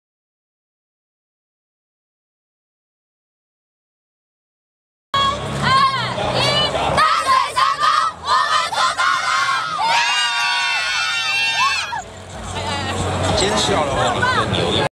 HBL淡商封后 永仁3連霸夢碎
HBL全國高中籃球聯賽女子組冠軍戰，今天由淡水商工對上尋求3連霸的永仁高中，淡商靠全隊六人得分上雙，硬是擋下衛冕軍最後的逆襲，最終奪下隊史第8座冠軍。攝影：張新偉